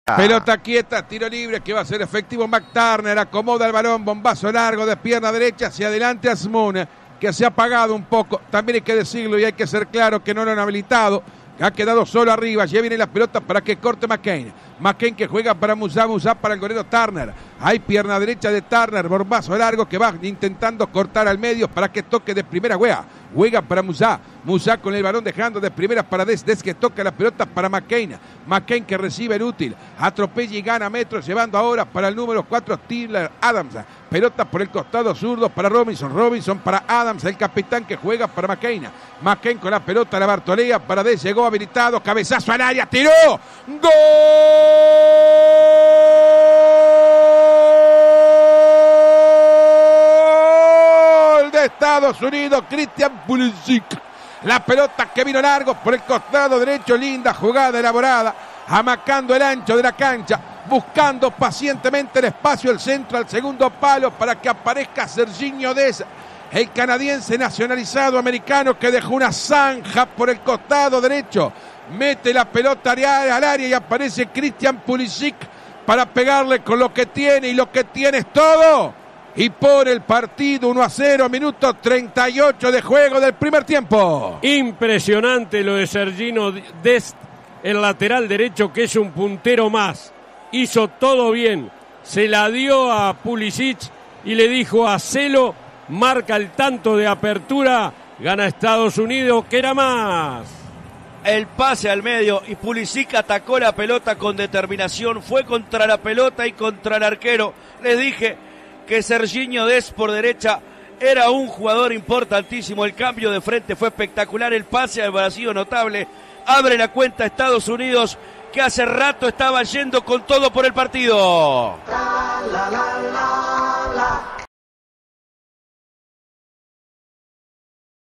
Irán 0 Estados Unidos 1 - Copa del Mundo 2022 (por Danielo Mera)
El gol del partido, por Danielo Mera junto a Ernesto Faría y Richard Arce para CX 22 Radio Universal 970 AM.